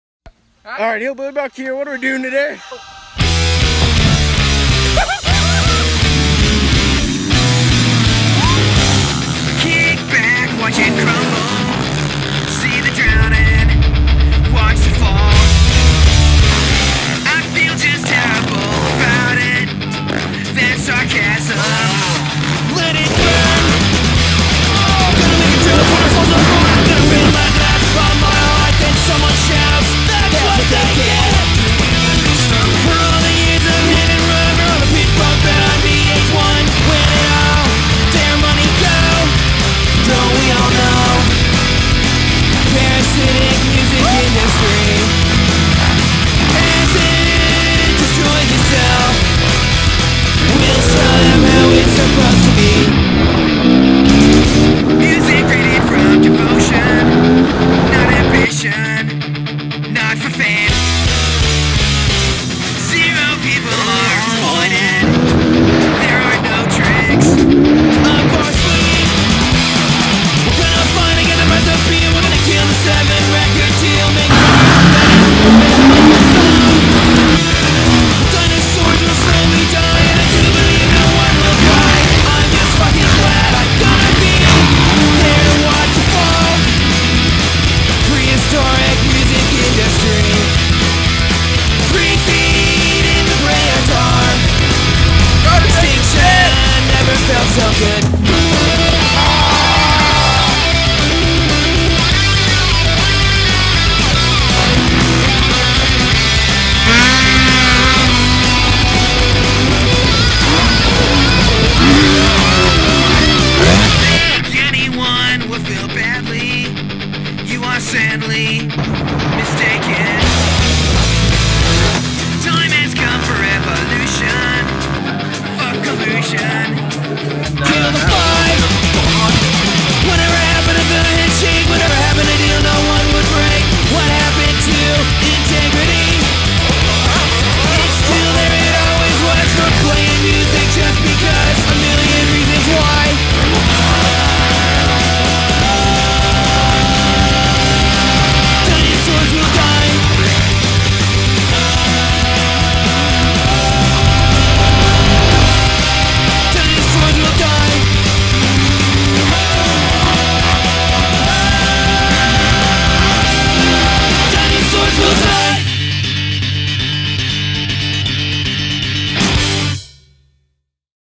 Moto moments of 2018
All my best footage i gathered through the 2018 motocross season. now that it has snowed i cut it together and through on some punk rock!

crack a beer and watch this shit!